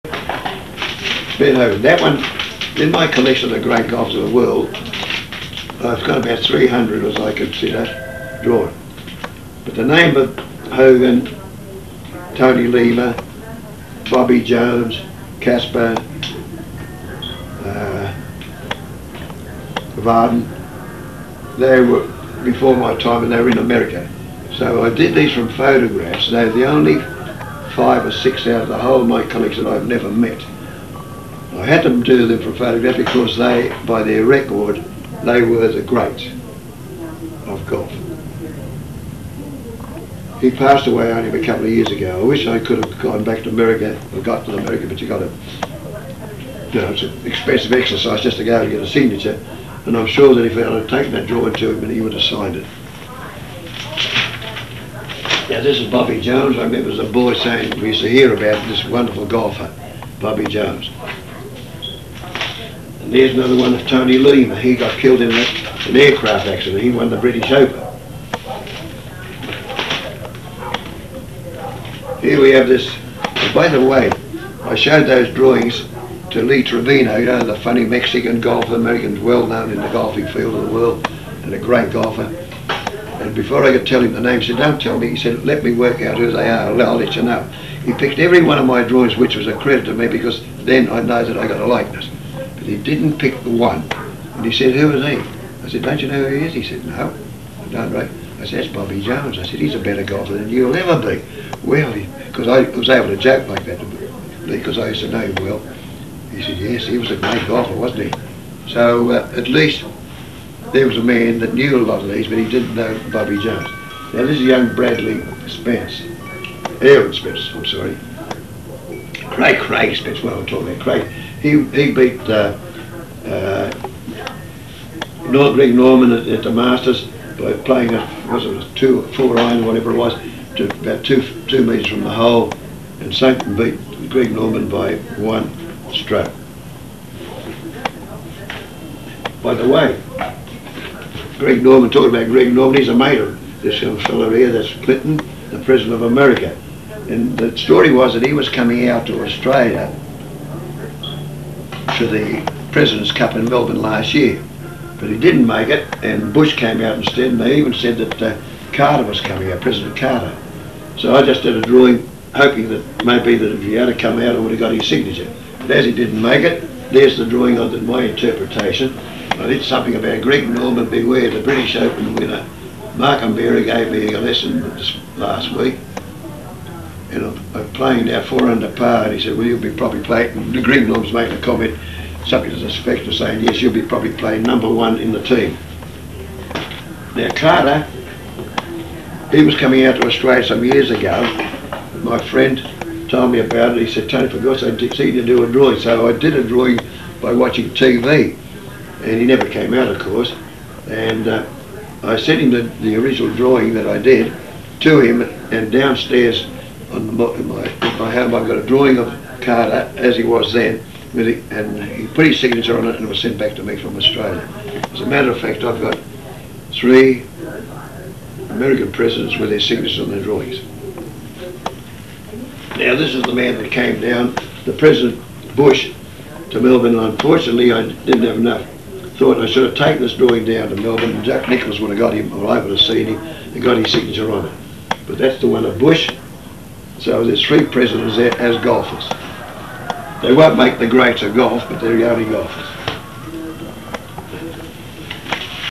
Tony Rafty Golf Caricatures Video 2
Australia's leading caricaturist Tony Rafty displays and makes comment on a selection of subject autographed golf caricatures from his Golf collection of more than 500 individually signed world's greatest golfers